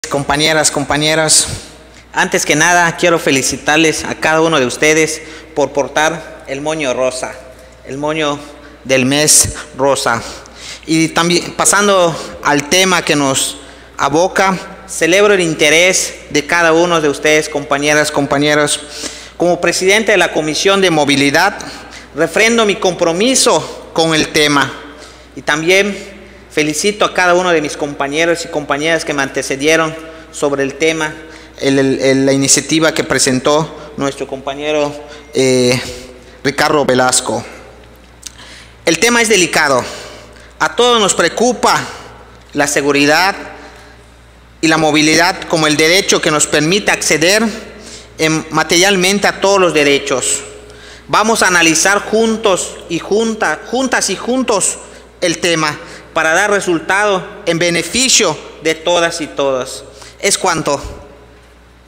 Intervención Dip. José María Chacón Chablé. MORENA (4/Octubre/2022)
Intervención del Diputado José María Chacón Chablé, de Movimiento Regeneración Nacional, en relación a la Iniciativa con proyecto de Decreto por la que se reforman y adicionan diversas disposiciones de la Ley de Movilidad del Estado de Quintana Roo y del Código Penal para el Estado Libre y Soberano de Quintana Roo; presentada por el Diputado Ricardo Velazco Rodríguez, Presidente de la Comisión de Puntos Legislativos y Técnica Parlamentaria del Grupo Legislativo del Partido MORENA de la XVII Legislatura del Estado.